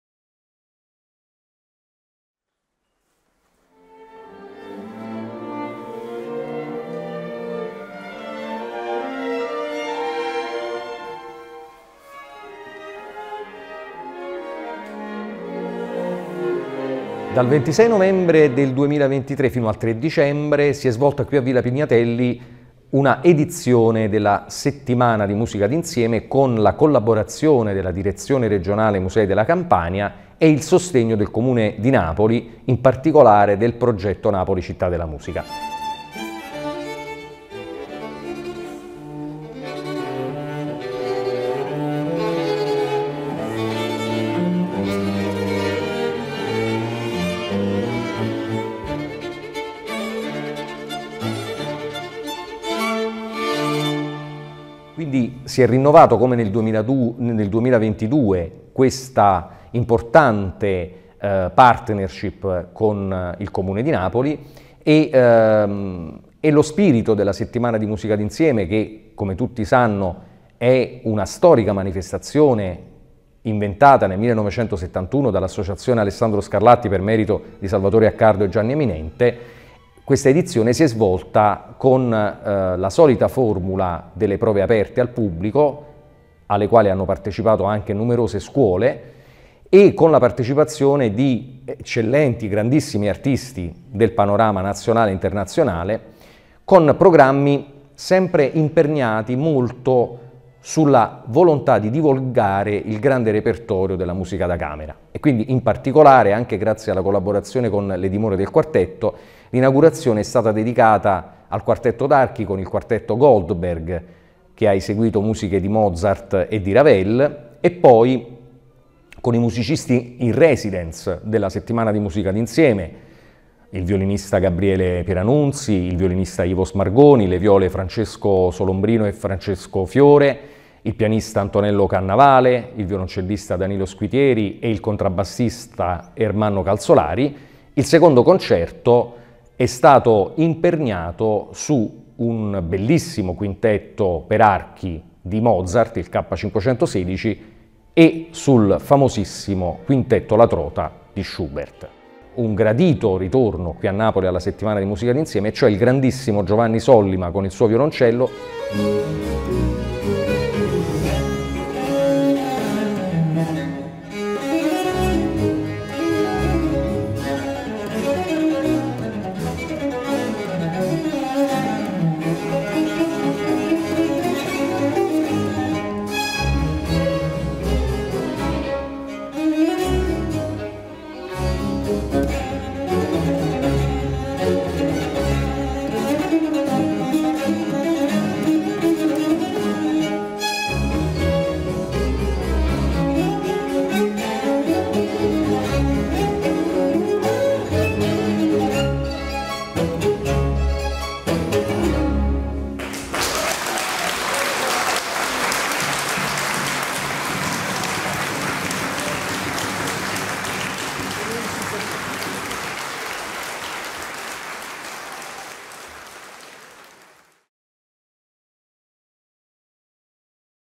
Settimana Musica d'Insieme 2023
Per l’anno 2023 l’Associazione Alessandro Scarlatti ha deciso di riproporre lo storico appuntamento della Settimana di Musica di Insieme, anche sulla base del successo dell’edizione 2022, rafforzandone alcune linee-guida. Il progetto, che prevede le consuete prove aperte al pubblico e i concerti nella Sala Veranda di Villa Pignatelli, trae forza dalla presenza di affermati cameristi del panorama internazionale, con il prezioso sostegno del Comune di Napoli e la collaborazione della Direzione generale Musei della Campania.